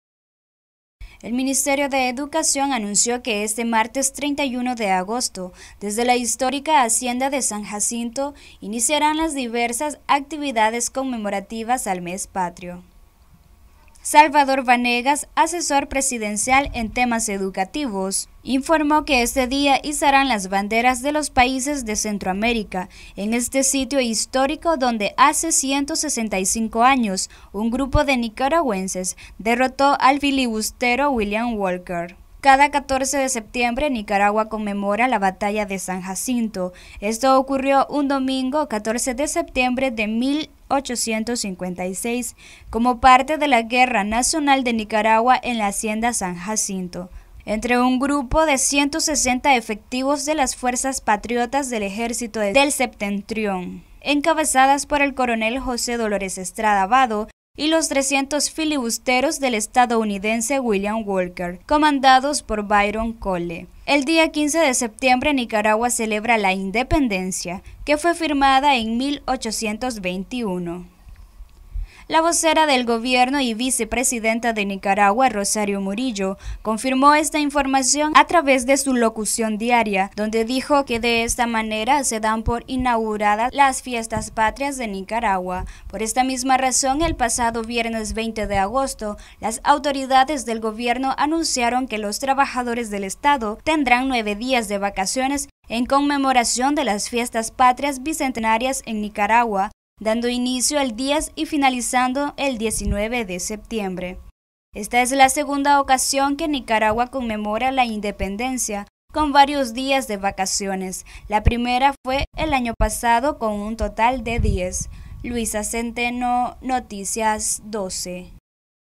Ministerio de Educación izará banderas centroamericanas en Hacienda San Jacinto
Noticias12Nicaragua Ministerio de Educación izará banderas centroamericanas en Hacienda San Jacinto este 31 de agosto para inaugurar fiestas patrias